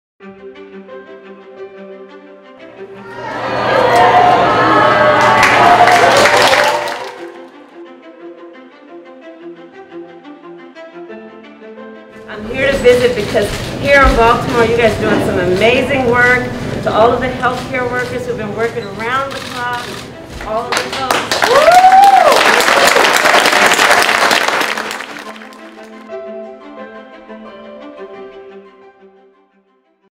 A VIP Visit at M&T Bank Mass Vaccination Site
Vice President of the United States Kamala Harris marks the 100th day of the Biden presidency with a visit to Baltimore and the mass vaccination site at M&T Bank Stadium.